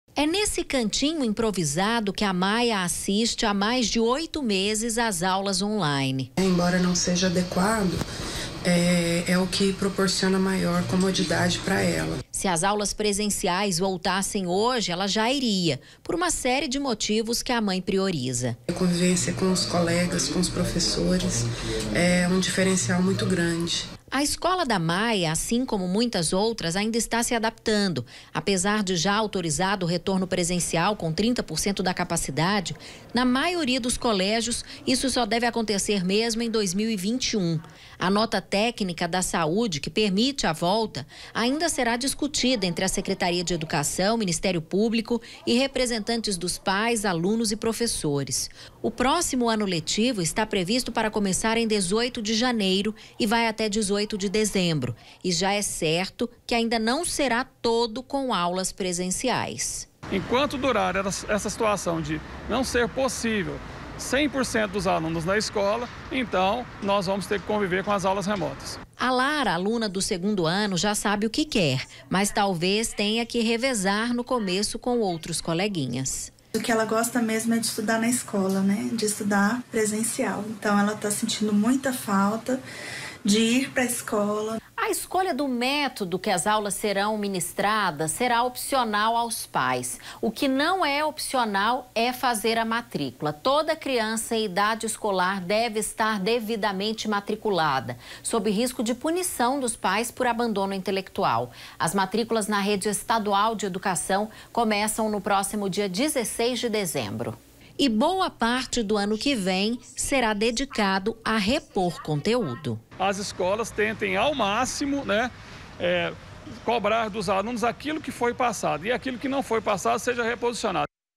ANO LETIVO DE 2021 AULAS ESTÃO PREVISTA PRA COMEÇAR EM 18 DE JANEIRO
A MAIORIA DOS COLÉGIOS OPTOU PELA VOLTA ÀS AULAS PRESENCIAIS EM JANEIRO. MAS OS PAIS QUE NÃO SE SENTIREM CONFIANTES VÃO PODER ESCOLHER SE QUEREM QUE OS FILHOS CONTINUEM RECEBENDO CONTEÚDO PELA INTERNET OU SE VOLTAM À ESCOLA. MAS INDEPENDENTEMENTE DA ESCOLHA, A MATRÍCULA É OBRIGATÓRIA. NO BRASIL, O ABANDONO INTELECTUAL É CRIME E OCORRE QUANDO OS PAIS DEIXAM DE GARANTIR EDUCAÇÃO  AOS FILHOS.